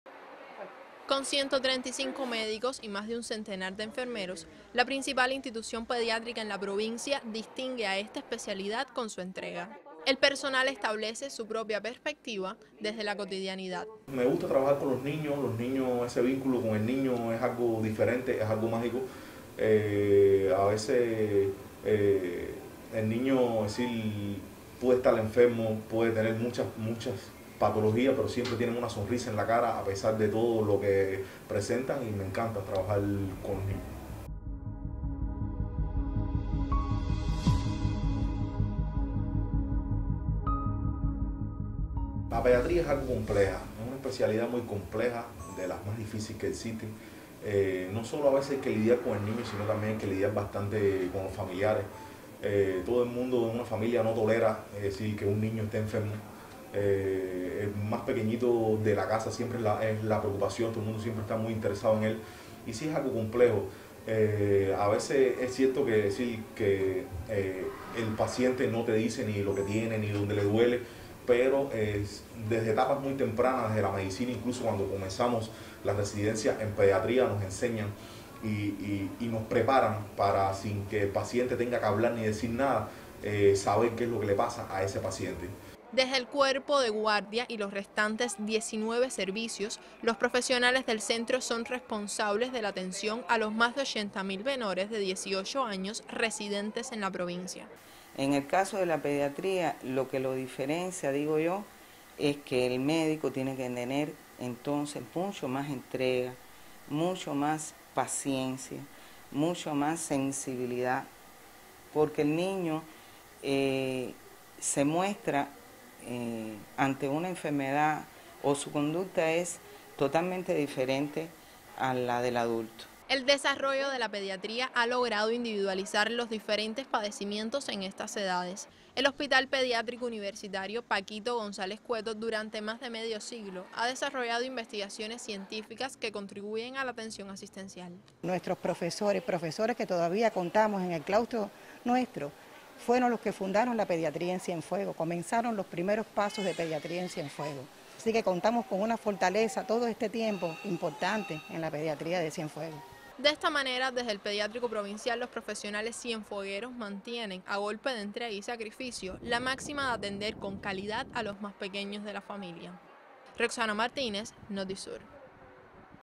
Pediatría cubana: una visión desde Cienfuegos.
Cuba celebrará este 30 de septiembre el Día del Pediatra, en honor a quienes diariamente cuidan la salud de los más pequeños. El Hospital Pediátrico Paquito González Cueto, de Cienfuegos, mantiene por 56 años la calidad de sus servicios.